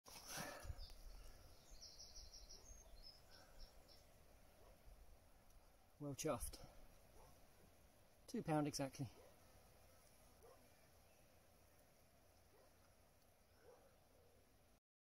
A real Goldfish! Rudd fishing
A beautiful Rudd caught on a Summers evening. One of several nice fish.